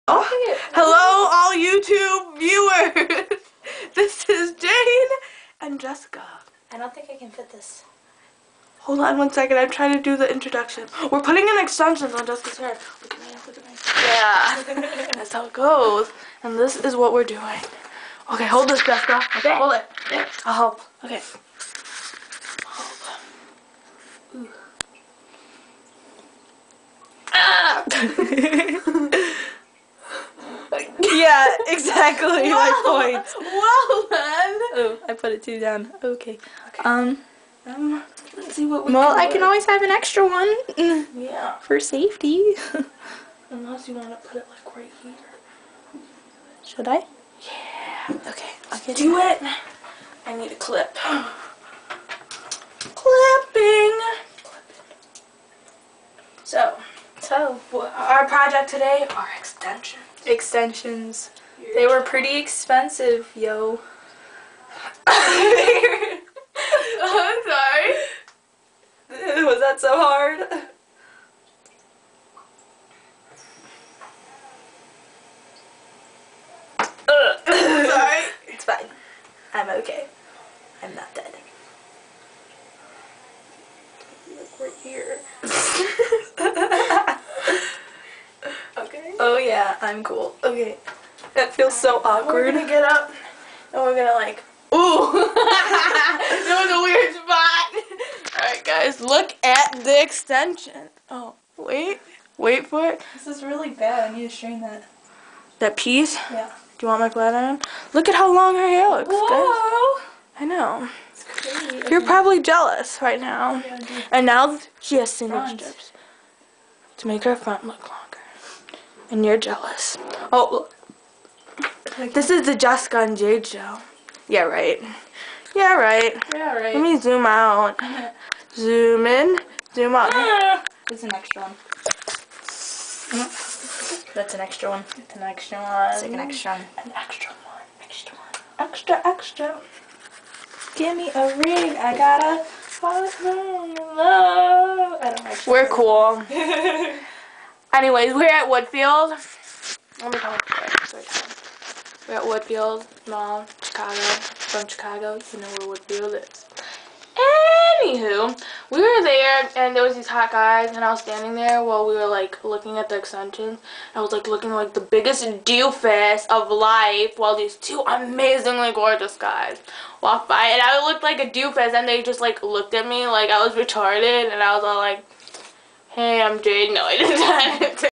Our First Video
Well Jess is Putting on Hair Extensions be jelouse